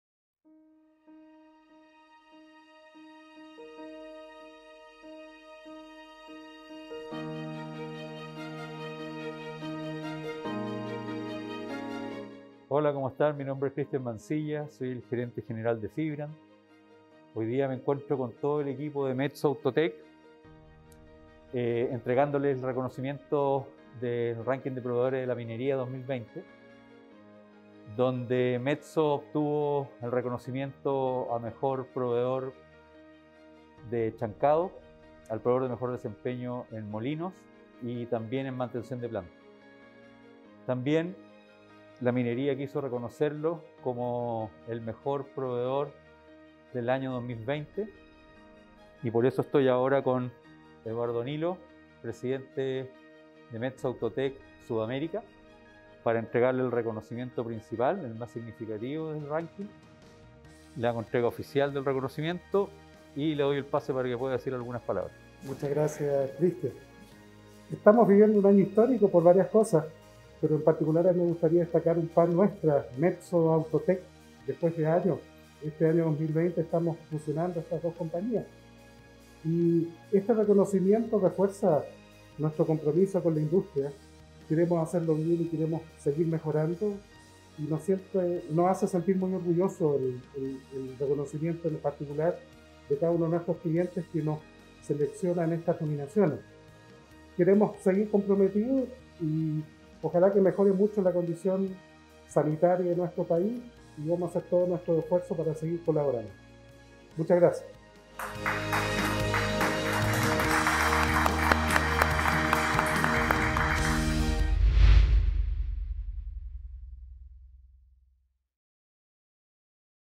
Metso Outotec receives recognition in Chilean Mining Suppliers' Ranking
Metso Outotec has received recognition as the leading supplier in the Mining Suppliers’ Ranking conducted by Phibrand in Chile. In the winning categories, Metso Outotec was nominated under the name Metso, as the survey was initiated in May 2020, prior to the merger of Metso Minerals and Outotec on July 1, 2020. These categories include Crushers, Mills and Plant Maintenance as well as the highest recognition in the ranking, the Best Performing International Supplier.